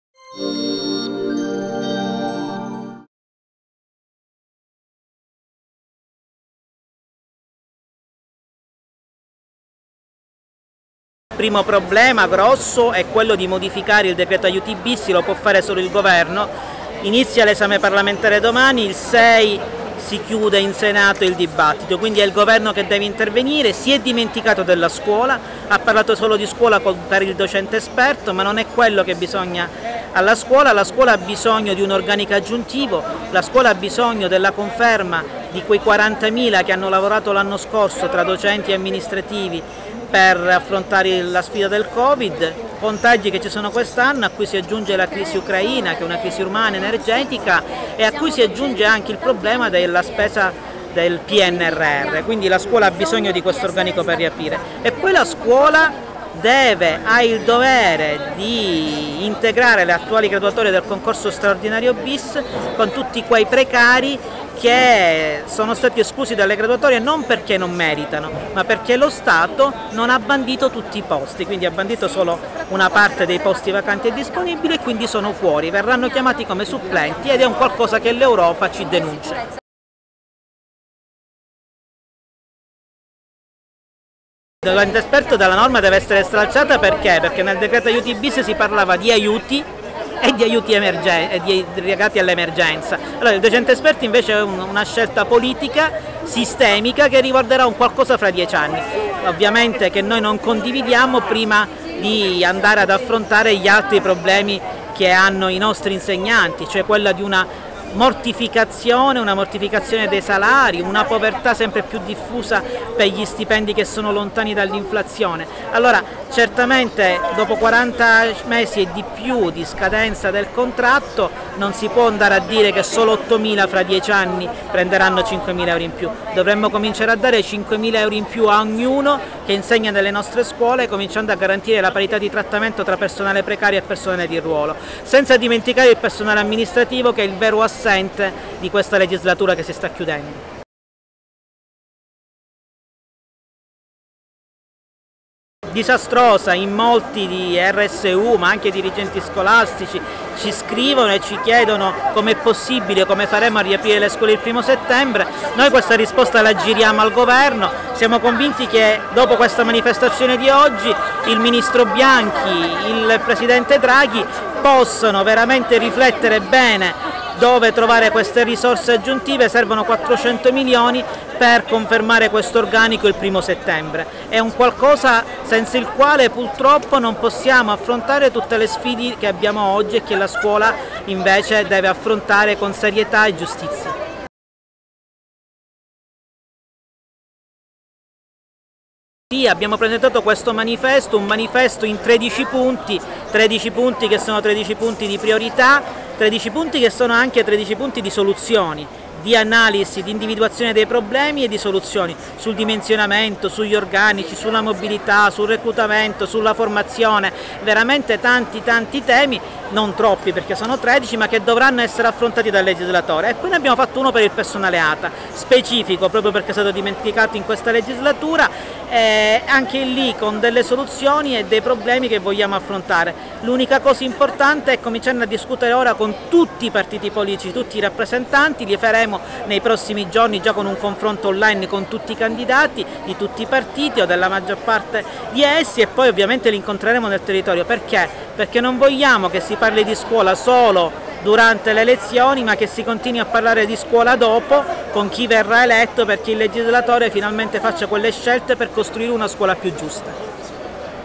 Ritorno in classe, Pacifico attacca: "Draghi e Bianchi si sono dimenticati della scuola"
Anief scende in piazza contro il governo Draghi. Intervista al presidente nazionale, Marcello Pacifico.

Intervista a cura di Patrizia Montesanti.